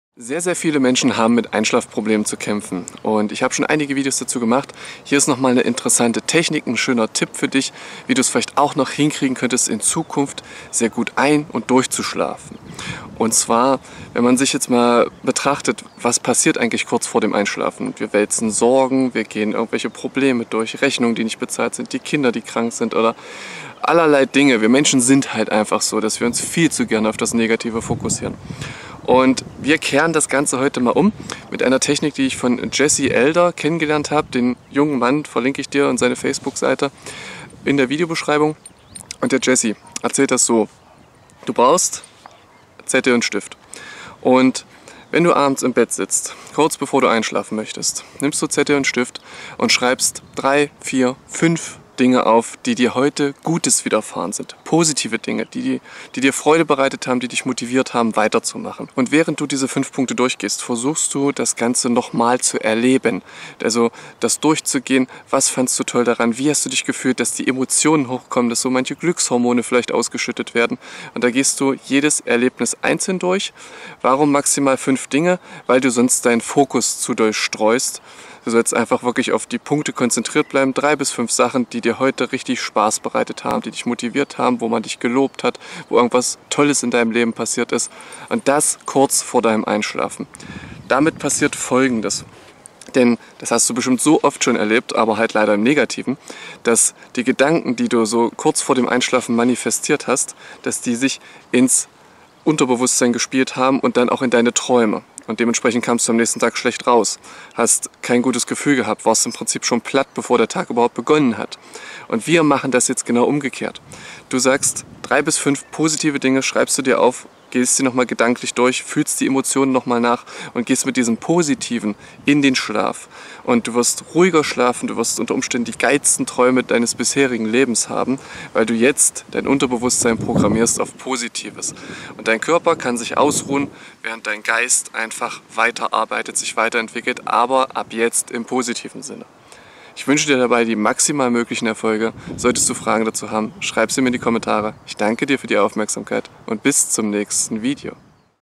Eine geniale Technik zum besser Einschlafen
Schneller und besser einschlafen. Ganz einfach, mit dieser simplen Technik.